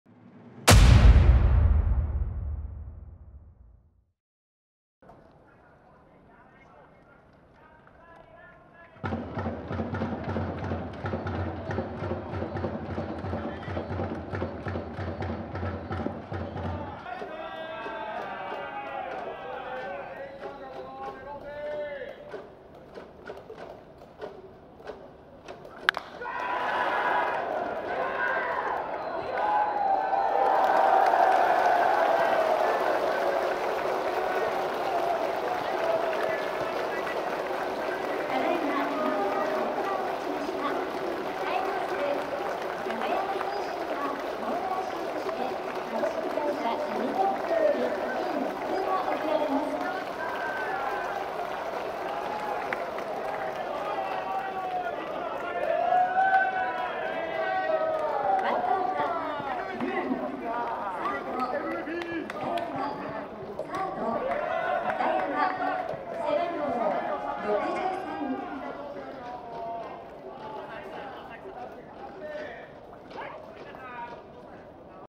1回表、髙山俊が先頭打者ホームラン！ -2022年プロ野球ファーム日本選手権-
2022/10/8（土）「2022年プロ野球ファーム日本選手権」
1回表、髙山俊の先頭打者ホームランで阪神が先制！

【試合情報】
10月8日（土）ひなたサンマリンスタジアム宮崎 13:00試合開始
J SPORTSにて生中継！